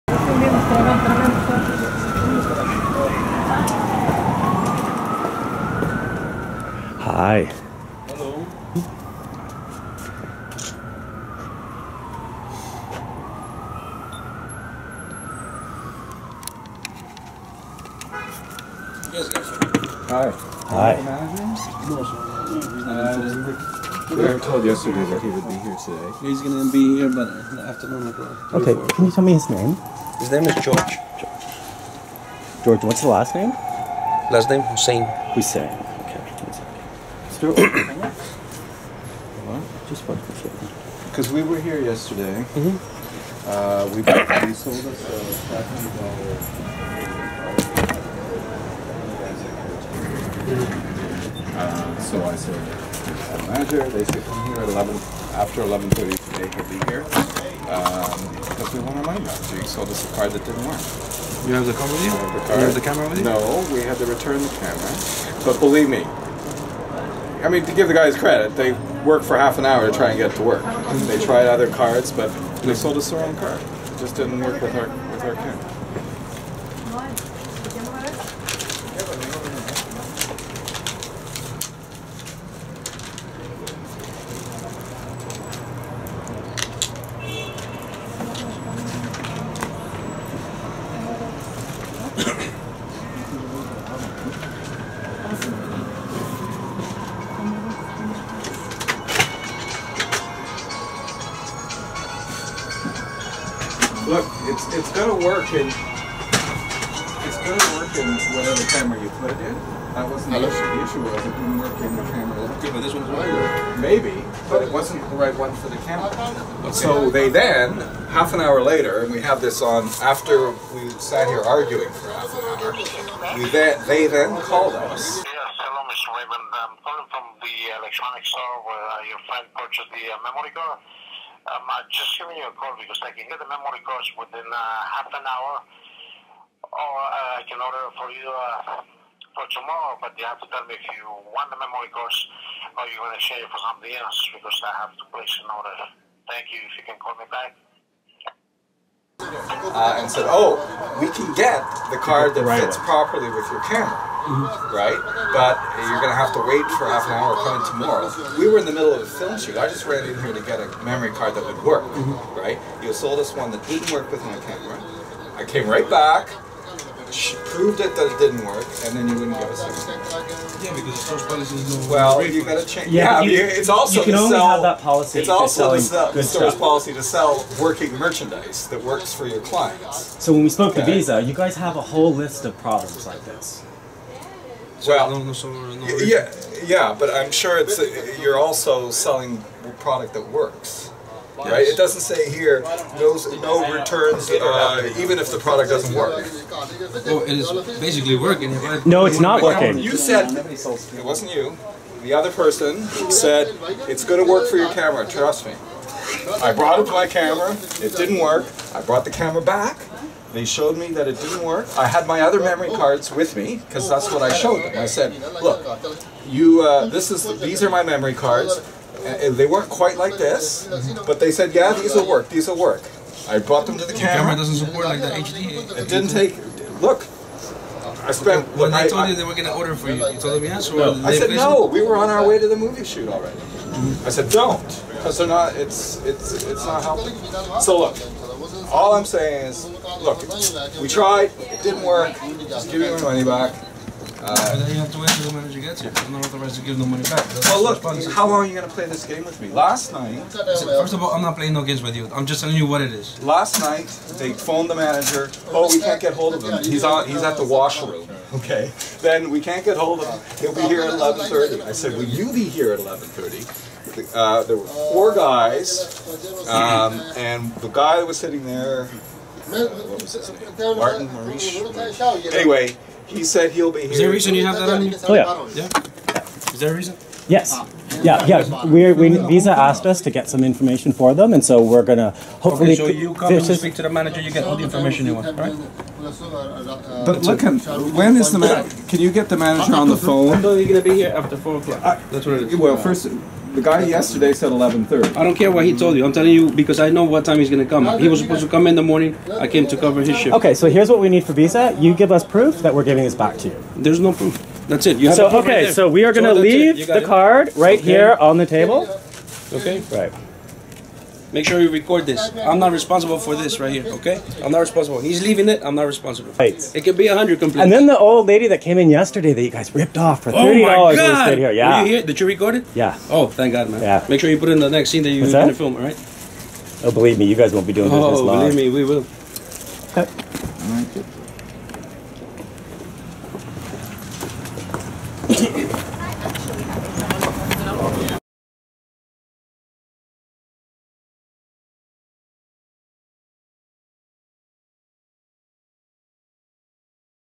SCAM: NY Camera and Electronics Corporation
UPDATE: After going through the Better Business Bureau and the NYC Department of Consumer Affairs, we took them to small claims court. We won.
This is our phone call to notify them as per the court judgment
Please sent CERTIFIED cheque to: XXX. If we do not receive it by Friday, we will initiate proceeding with the City Marshal's office. Please confirm receipt.
Tuesday/Wednesday: They left several messages for us to call in our credit card info so we emailed::
Wednesday September 2 we emailed: We cannot provide you with credit card information, you can pay by certified cheque or money order. You can make other arrangements provided the money is received by us on or before Friday September 4th, 2015. Do not send mail requiring a signature as this address below is for postage only. Best,
Thursday September 3, 2015: "Jhon Smith" replied to our email with a photo of an International Money Order addressed to us.
BACKSTORY:
We were sold a media card for $450 that he guaranteed would work on my camera. I had the type I needed with me and instead he sold me an over-inflated media card that was for a different camera. When we went back to the store 10 minutes later to return the card the games started. He told us no refunds.He gave us three different names for the manager and several different times that he would be there, but wasn't. Of course after we've been ripped off we saw all the complaints online of the exact same thing they've done to other people.

How can NYC allow tourists and New Yorkers to get ripped off like this. I went by the store today and there was another lady there screaming cause he's ripped her off and ruined her trip.

NY Camera and Electronics Corporation
Lic. 1232688, 1232026